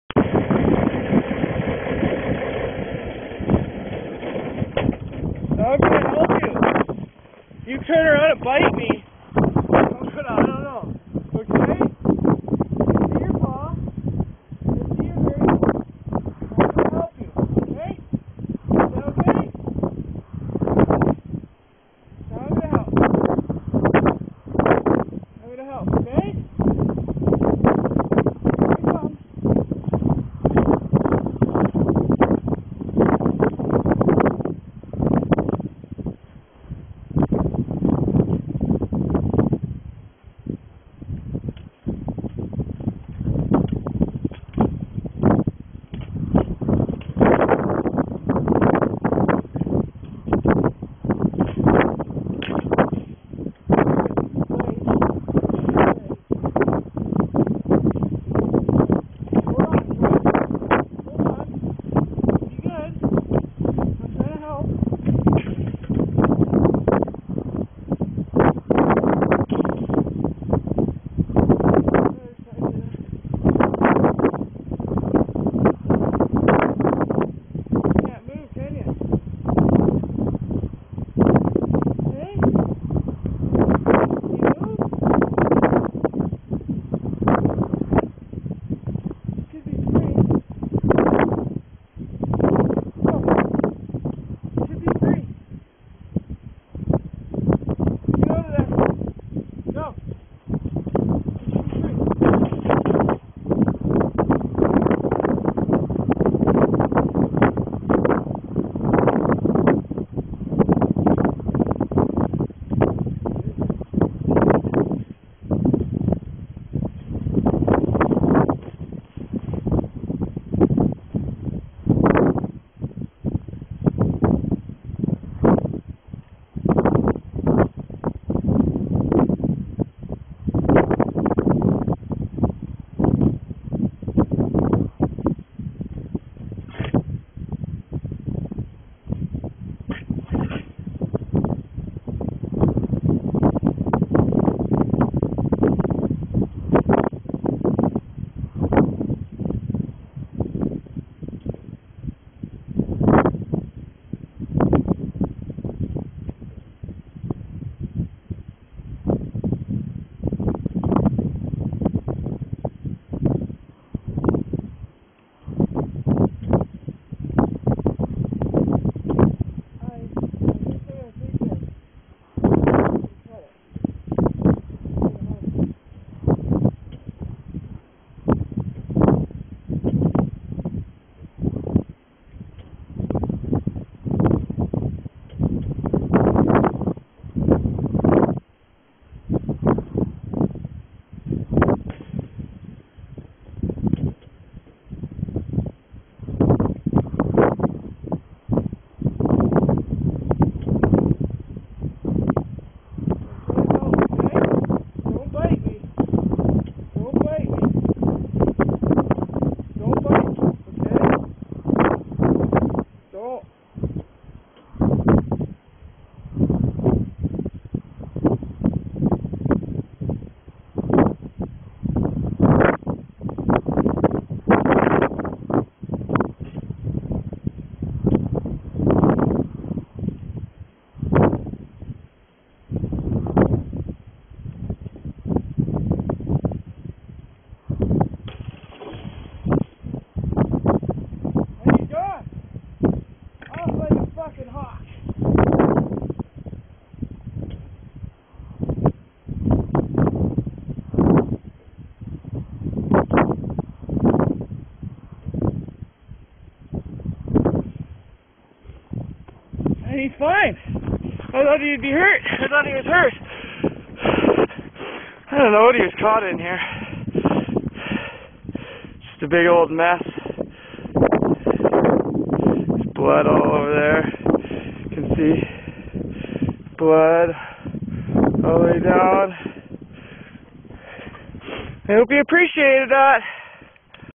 Wolf Saved From Barbed Wire
Hikers came across a wolf stuck in a fence and decided to free it from barbed wire.